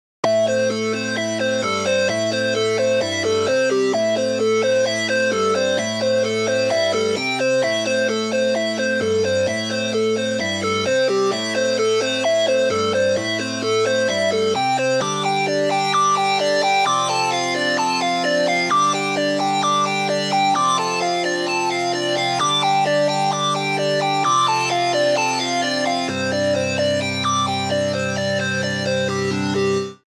What if: Psychic Island had a Mythical? || Amphiloo (V2)
Element: Mythical
Class: Mythical
Breeding: Periscorp + Rootitoot
Teleports: Mythical Island
Instrument: Amplified Synth

Bio: From across another dimension, a mecha suit was dropped onto Psychic Island, with the brand name on the lower side reading “Blabo Tech”. It appears that Blabo has invented a mech and sends it into space to test and see if it works. After observing the robot, it is discovered that the robot’s name is Amphiloo which is a mech like chamber that stores in these mysterious creatures known as “Tripods.” These creatures’ origins are unknown, but they are endangered species, so that is what the Amphiloos were designed for. Keeping and protecting these creatures, making Amphiloo a great guardian to these cute little Tripod fellas.

Portions of the materials used are trademarks and/or copyrighted works of Big Blue Bubble. The materials are not official and are not endorsed by Big Blue Bubble.